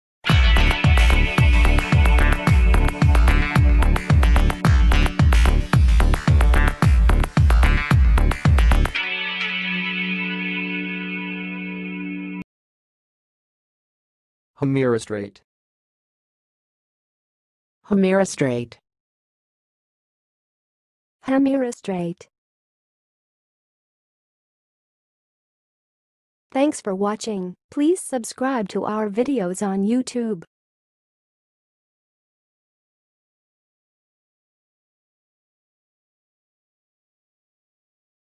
How to say "hamirostrate"! (High Quality Voices)
Watch in this video how to say and pronounce "hamirostrate"!